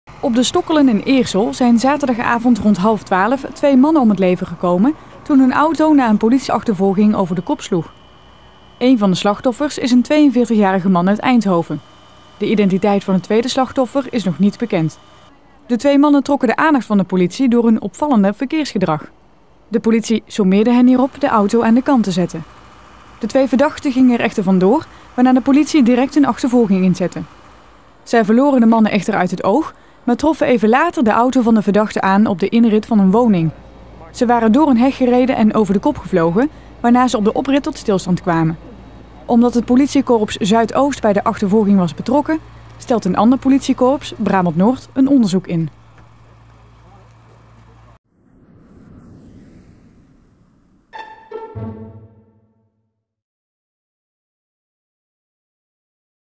Doden na achtervolging politie
Op de Stokkelen in Eersel vielen twee doden na een achtervolging